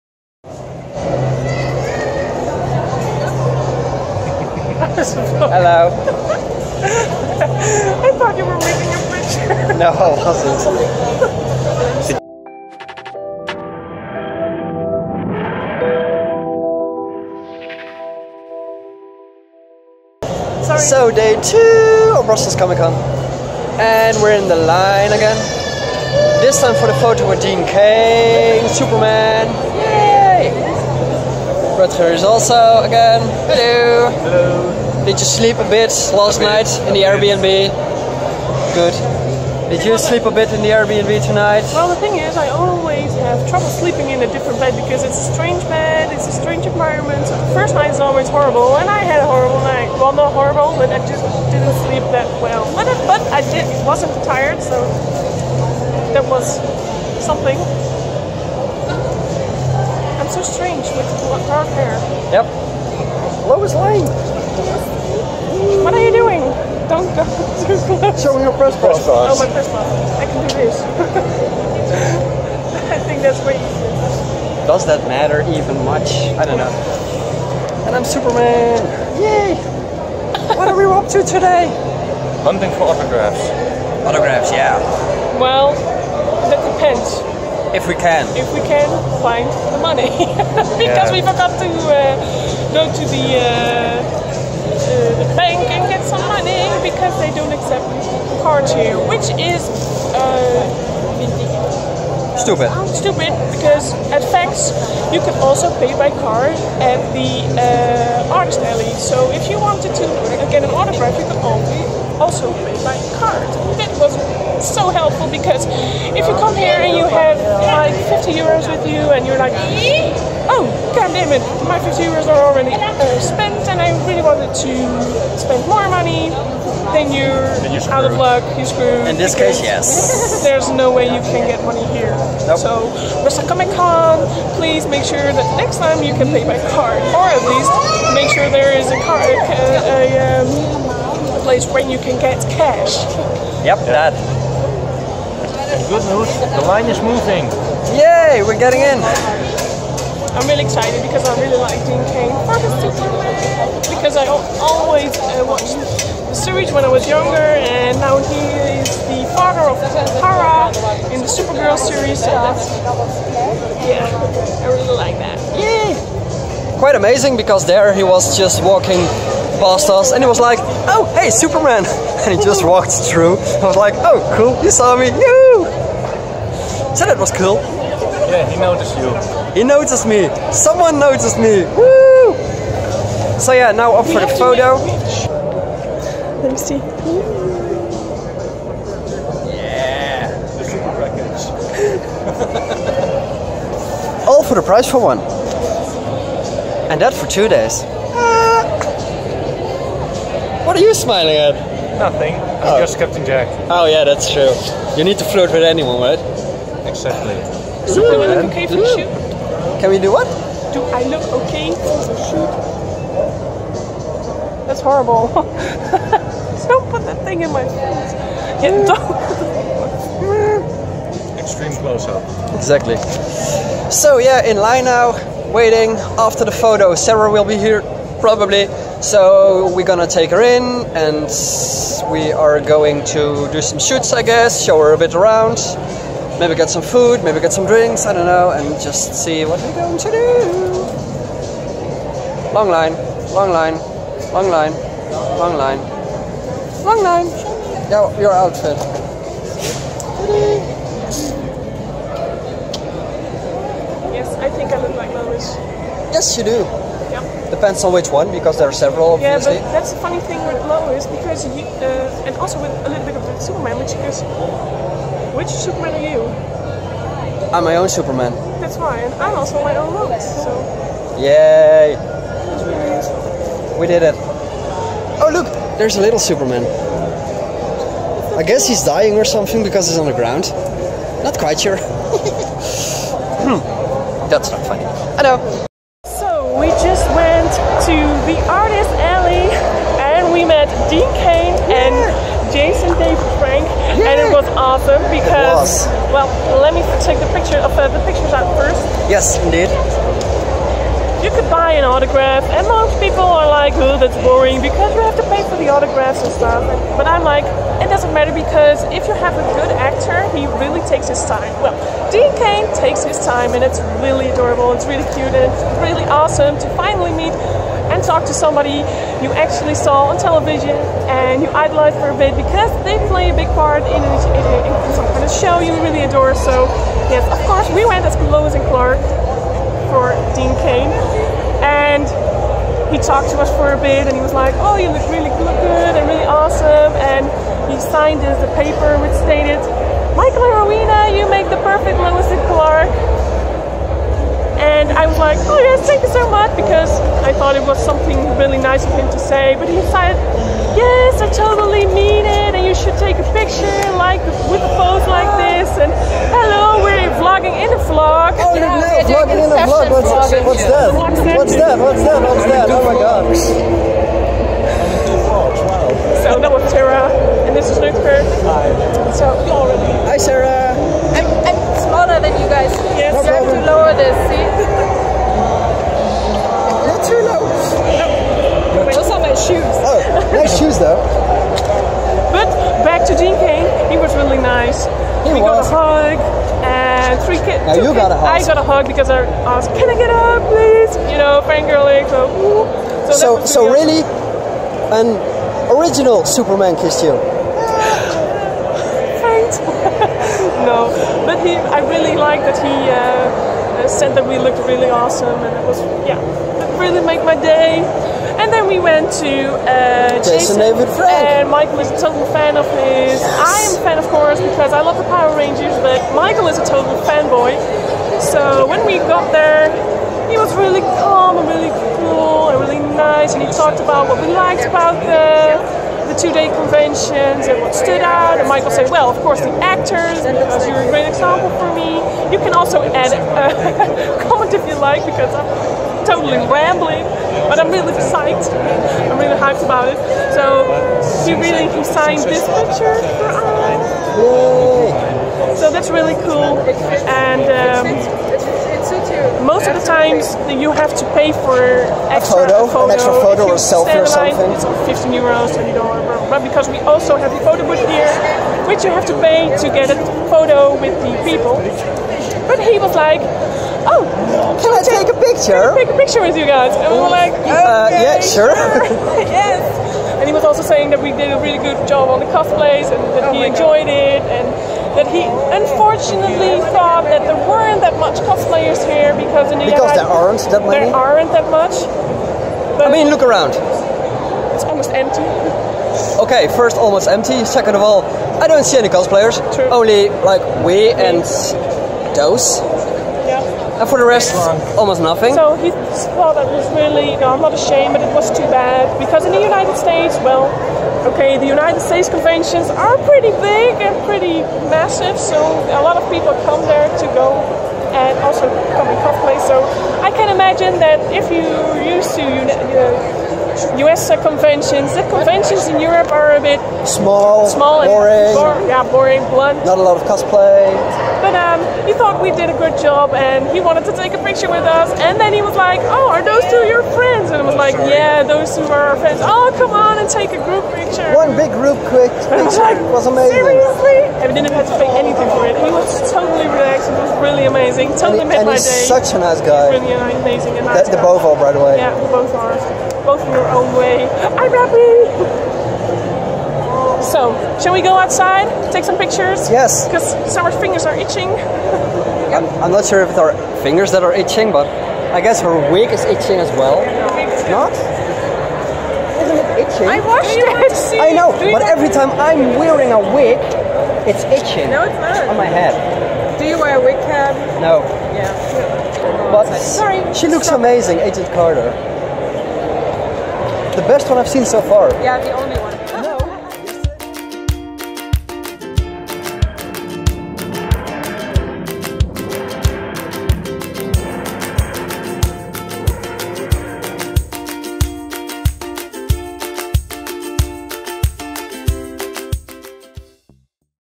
Second day Brussel Comic Con [ 11 Feb 2018 - Day 42 ]
Second day of Brussel Comic Con. Meeting Jason David Frank and Dean Cain. What an amazing day! What an amazing weekend!